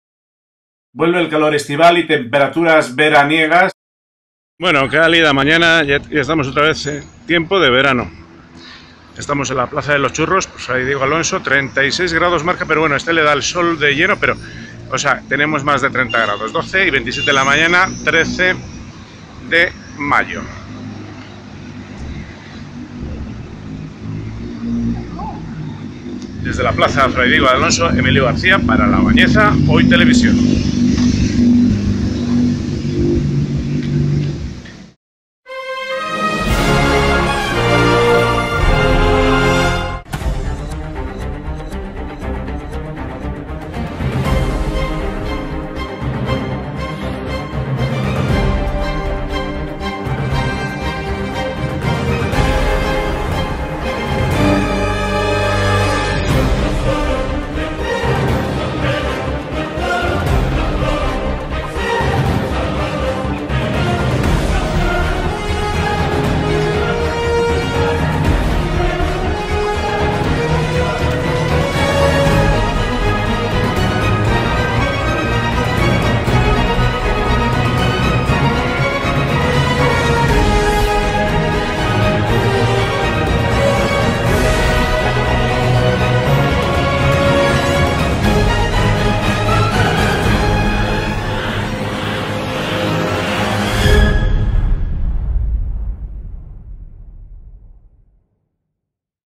Vuelve el calor
Informativo local
Tras lluvias y tormentas, volvemos a superar los 30 C